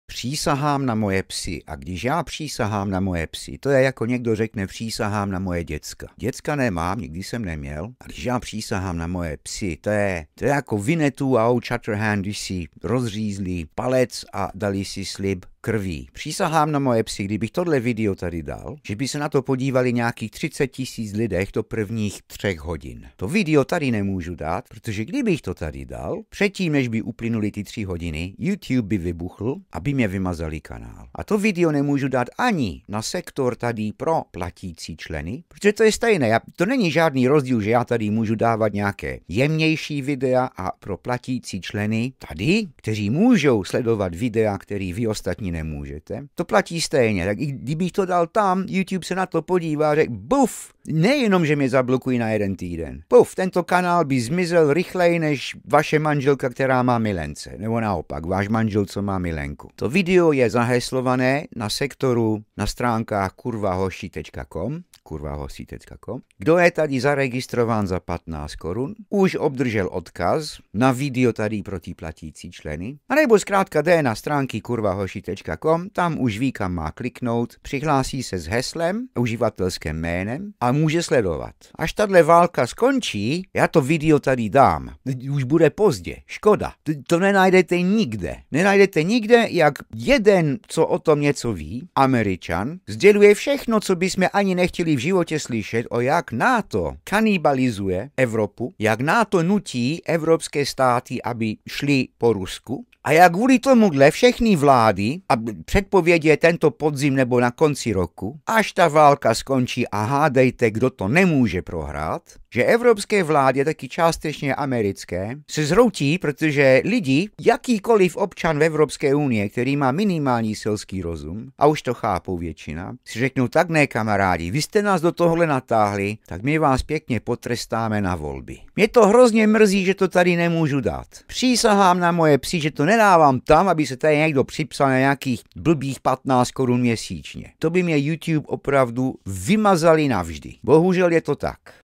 NATO KANIBALIZUJE EVROPU A DONUCUJÍ EU STÁTY ABY BOJOVALY PROTI RUSKU. VLÁDY EU DOPADNOU ŠPATNĚ
Někdo kdo tomu rozumí akorát zdělil pravdu o NATO a jak EU vlády nedopadnou dobře až bude konec války. Voliči je všichni pošlou do háje včetně Fiala a jeho kumpány. Video je na